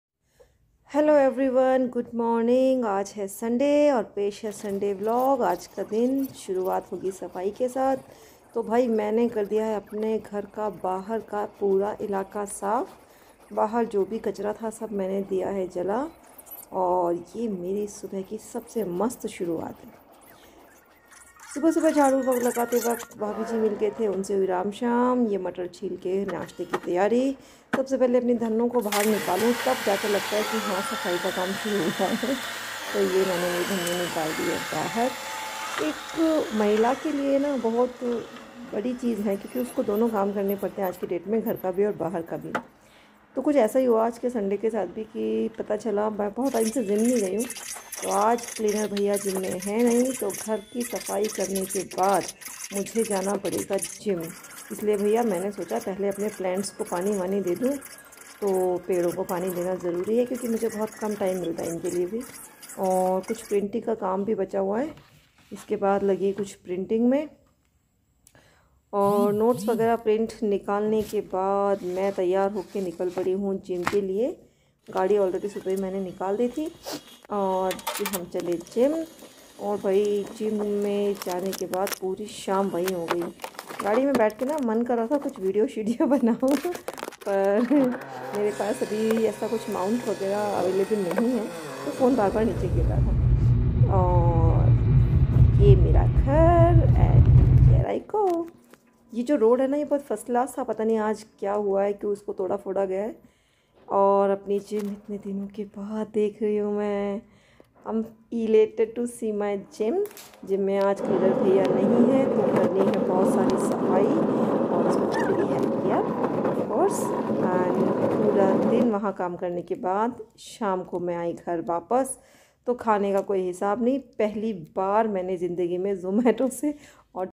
My sunday volg#002
sunday vlog ...a very hectic day today sara time aaj gym k kaam me hi nikal gaya